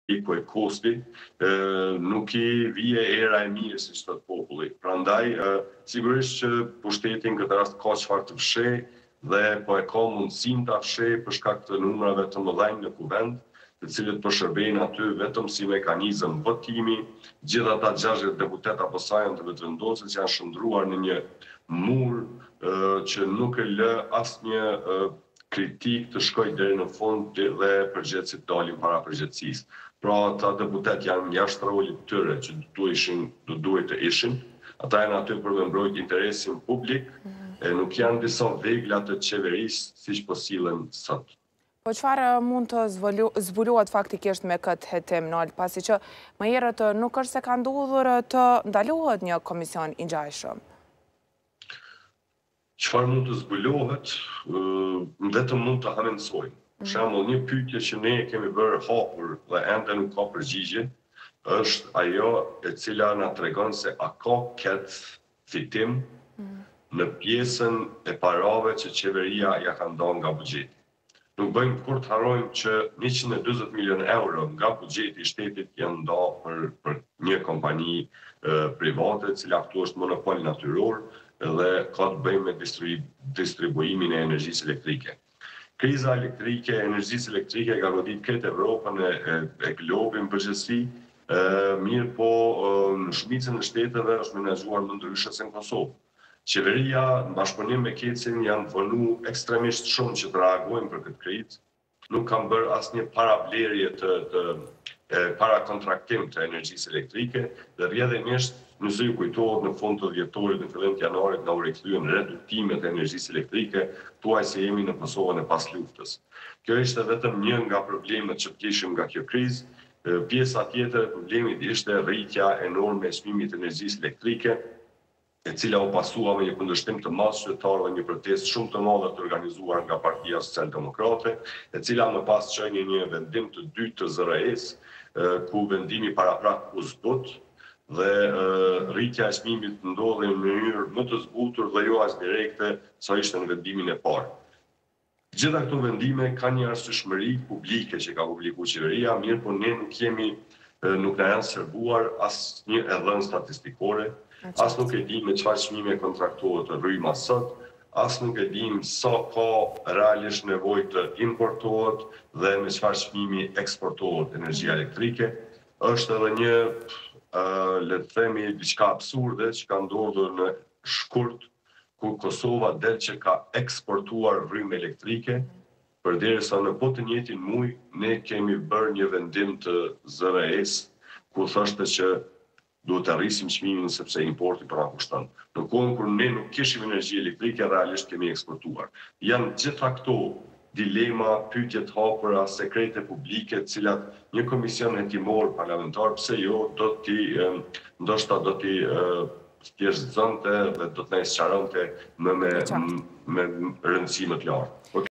Nushi: KEDS-i ka fitim nga subvencionet e Qeverisë
Lamih Balaj - Kryesindikalisti i Telekomit
Nol Nushi - Partia Socialdemokrate
Halil Krasniqi - Ekspert i shëndetësisë dhe politikan në Gjermani